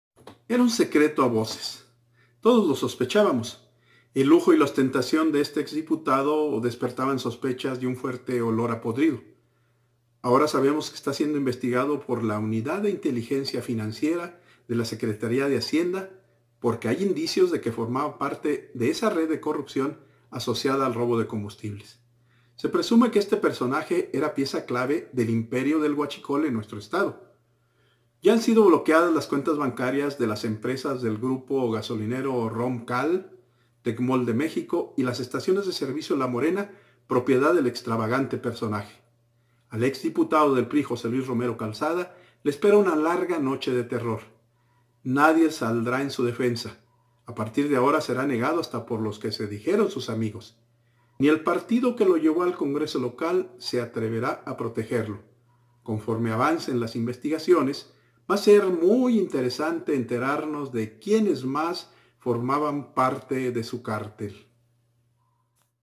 JOSÉ LUIS ROMERO CALZADA Y EL IMPERIO DEL HUACHICOL.
EN LA OPINIÓN DEL LIC. ÁNGEL CASTILLO TORRES